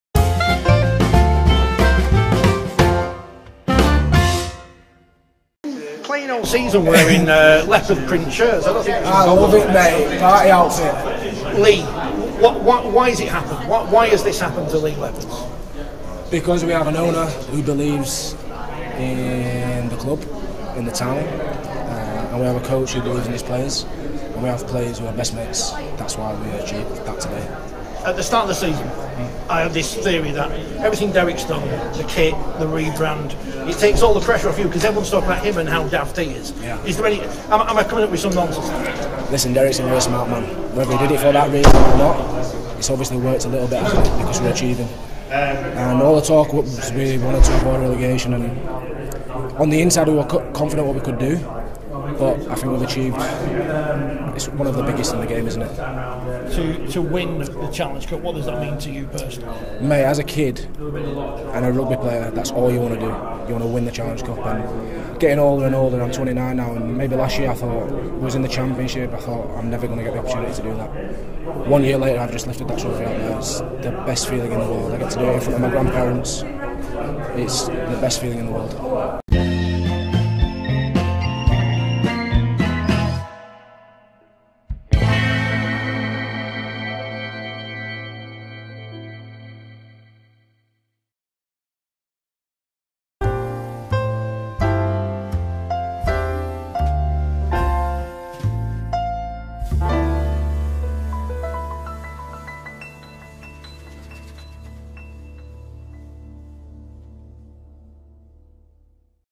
Ben Reynolds on Leigh's simple formula to success - Forty20 TV
Leigh Leopards are the 2023 Challenge Cup winners, beating Hull KR in Golden Point 17-16.

Ben Reynolds joined us in the tunnel afterwards, as we asked him how it happened.